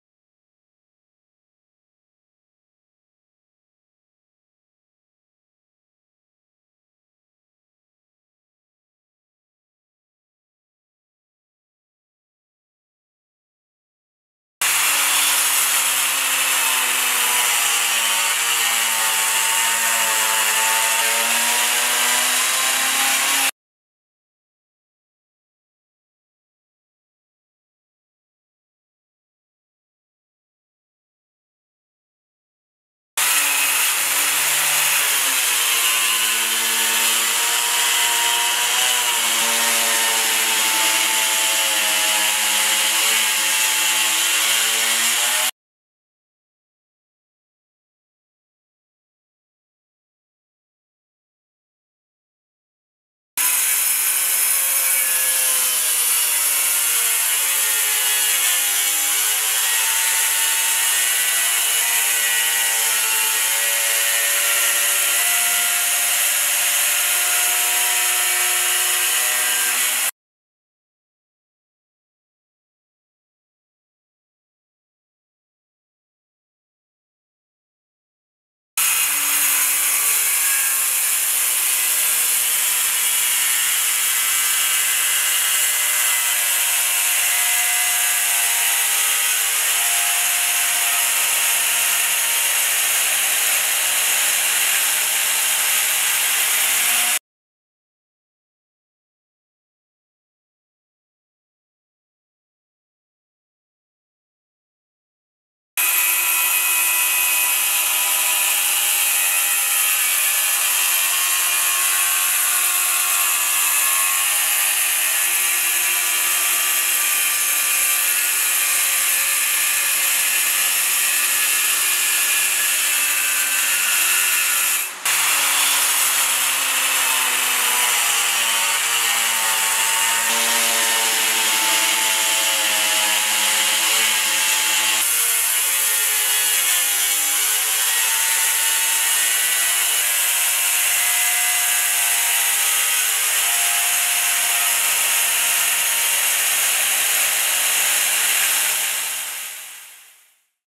TEST TARCZY DIAMENTOWEJ EXTREME TOOLS KERAMIK
Tarcze diamentowe Extreme Tools Keramik przeznaczono do obróbki twardych materiałów: ceramiki, gresu, klinkieru, cegły, suporeksu, kamienia, granitu, marmuru oraz marmuru. W narzędziach zastosowano wzmocniony i wyciszony korpus typu sandwich, co pozwoliło zmniejszyć grubość segmentu tnącego do 1,4 mm.